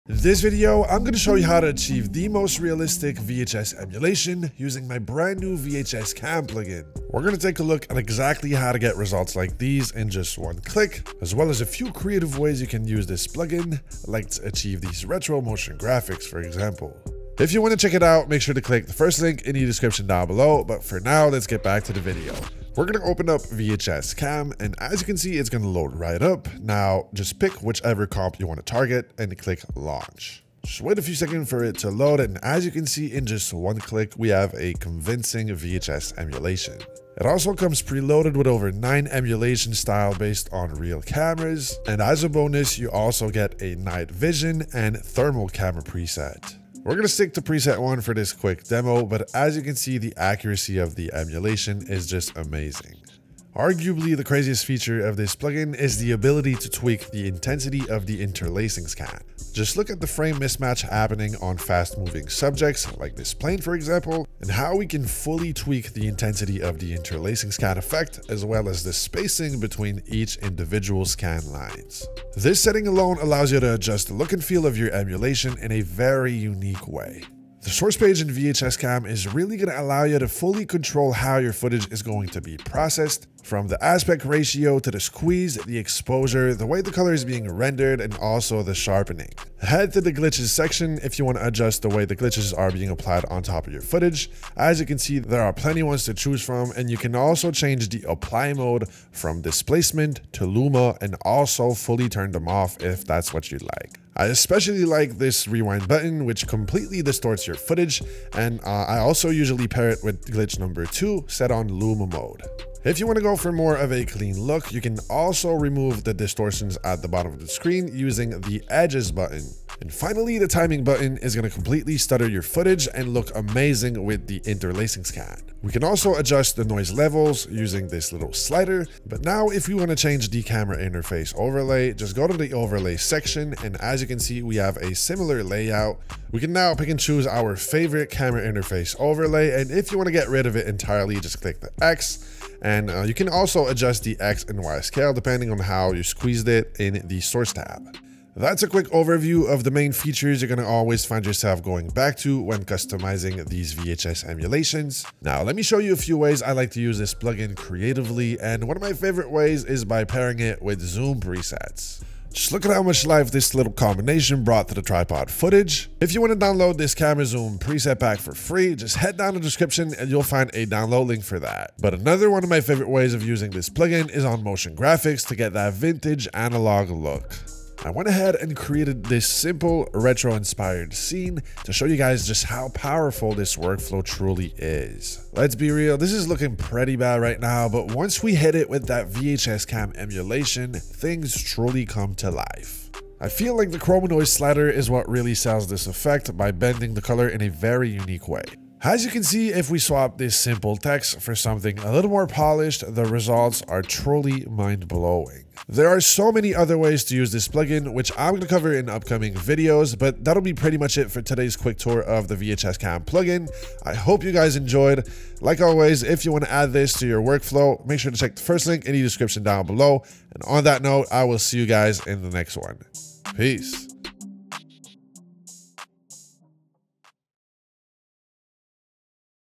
VHS Emulation in One-Click (+ FREE Pack)
Learn how to emulate the VHS / Camcorder look in One-Click using VHScam for After Effects.